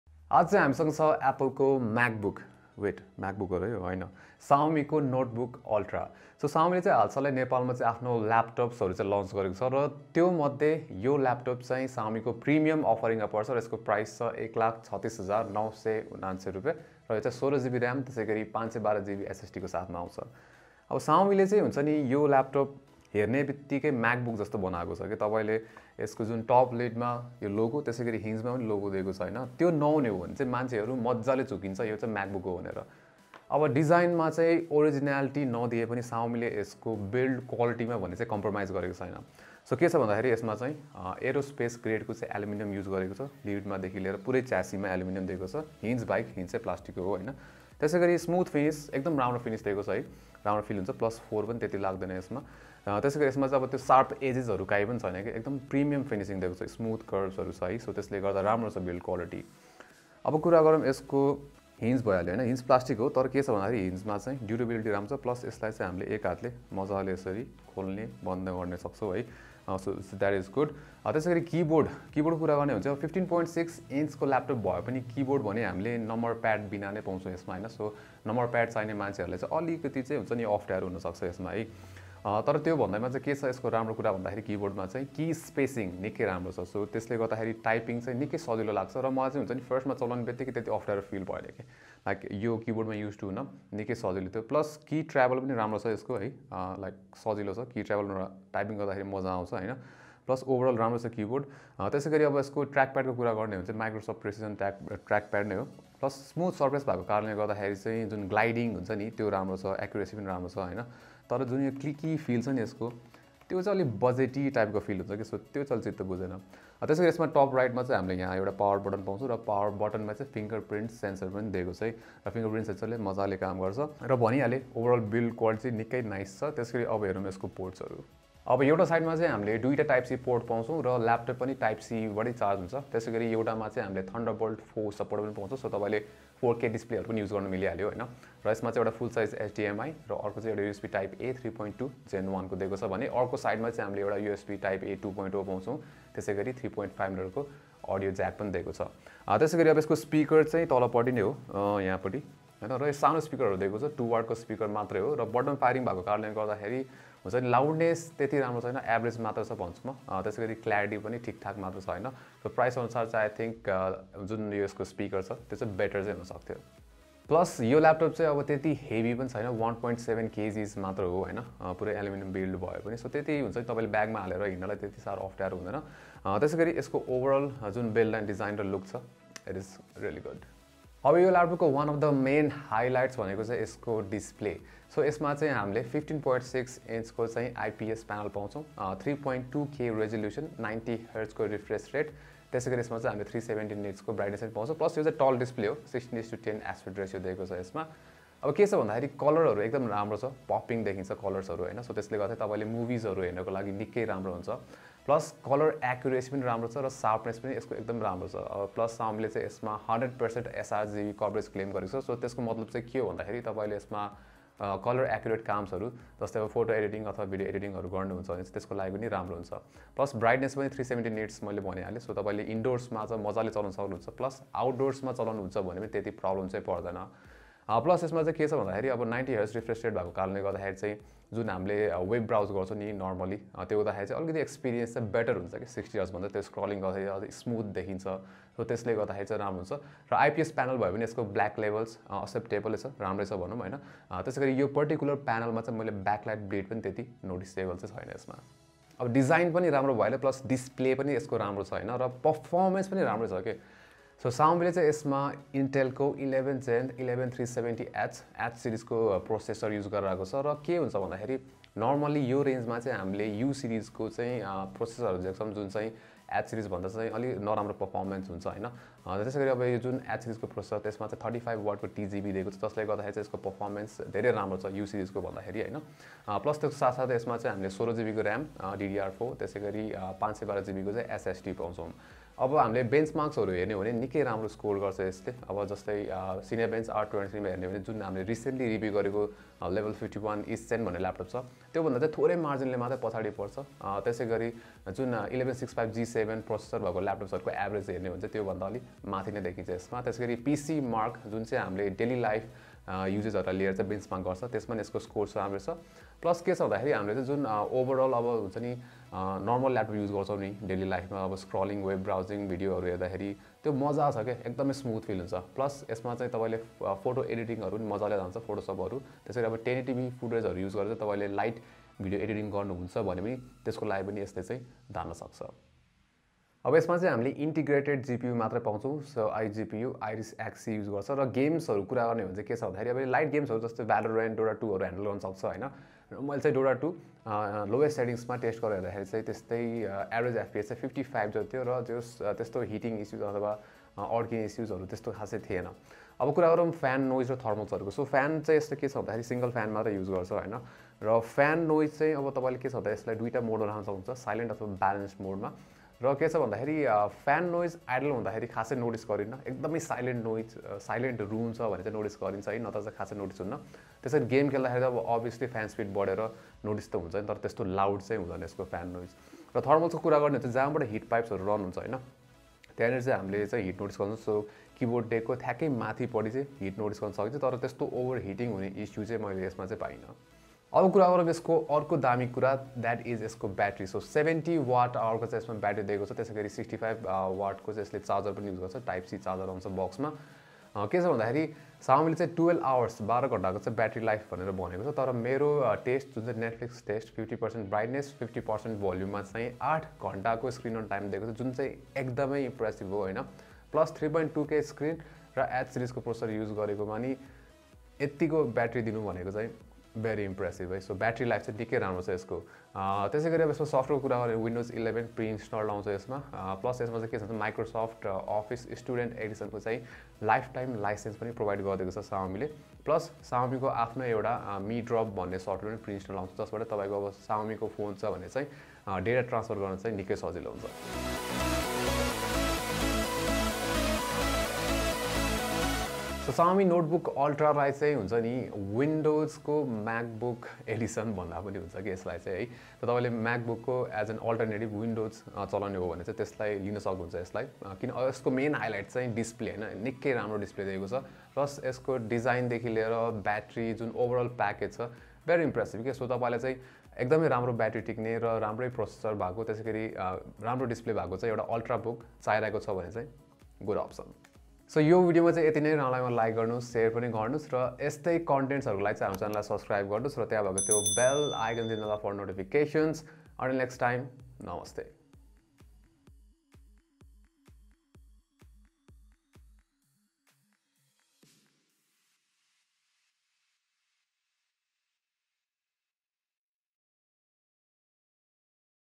Mi NoteBook Ultra Review नेपालीमा: Best Laptop under 1,50,000 in Nepal?
Want to buy a Mi NoteBook Ultra laptop in Nepal? Is it the best laptop under 1,50,000 in Nepal? Wait! First, watch our Mi NoteBook Ultra review video!

We hope you will like this Mi NoteBook Ultra review video in Nepali. Also, make sure to watch it till the end.

Mi NoteBook Ultra Overview and Specs
Mi NoteBook Ultra Price in Nepal: Rs. 1,36,999 (16GB RAM + 512GB SSD)

Xiaomi has made the Mi NoteBook Ultra look like a MacBook. If not for the Xiaomi logo on the hinge and lid, one could easily mistake the Mi NoteBook Ultra for a MacBook.

While the NoteBook Ultra doesn’t have an original design, it is built well with premium materials.

It is totally made out of aerospace-grade aluminum which makes it moderately light at 1.7 kg.

Mi NoteBook Ultra’s 15.6-inch 3.2k display was a treat to my eyes. The 90Hz IPS panel with a 16:10 aspect ratio has vivid colors and it is plenty sharp.

Not only is it bright but it has 100% sRGB coverage which makes it a good option for photo and video editors. It does have good color reproduction and I enjoyed streaming videos on this device.

Mi NoteBook Ultra uses an Intel Core i7-11370H processor, a quad-core processor with 8 threads, and a base clock of 3.3GHz.

This is paired with 16GB RAM and 512GB NVme SSD, the latter can be upgraded as the RAM is soldered.

The NoteBook Ultra has a 70Wh battery and it lasted for 8 hours in my Netflix test with 50% brightness and 100% volume. For charging, it gets a 65W USB Type-C charger.

In terms of the speaker, it has a bottom-firing speaker setup on the bottom of the laptop. The speaker has good clarity but it could have been louder.

The NoteBook Ultra is built well and looks like a MacBook. So, if you want a windows laptop that looks like MacBook, this is the one. On top of that, it has good enough performance, a good display, and a long-lasting battery.

What do you think about the Mi NoteBook Ultra?